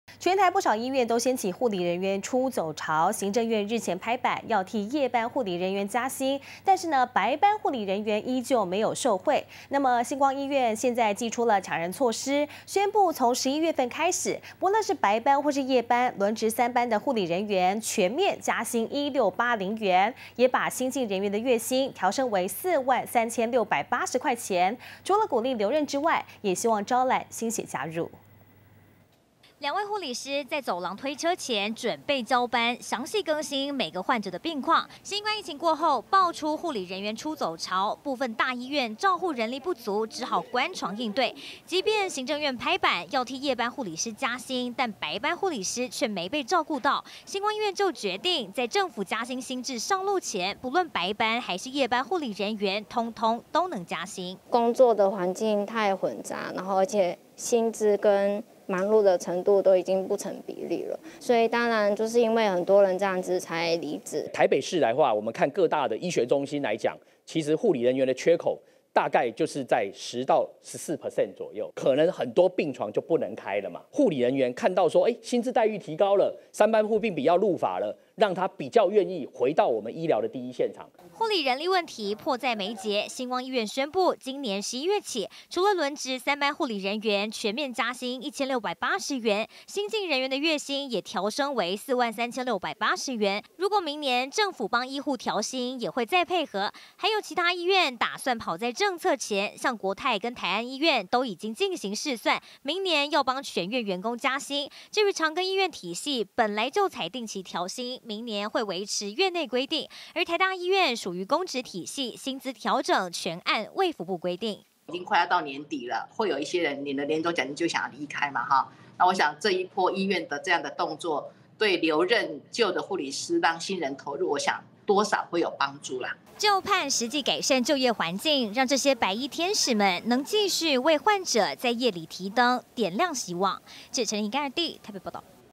搶先政府加薪！ 新光醫院輪3班護理人員多1680元
全台 不少醫院，掀起護理人員 出走潮，雖然 行政院日前拍板，要替 夜班護理人員 加薪，但 白班護理人員 依舊沒受益。新光醫院搶先祭出搶人措施，今天(10/27)宣布，11月起，輪值三班的護理人員，全面加薪1680元，也調升新進人員的月薪，除了鼓勵留任，也要招攬新血加入。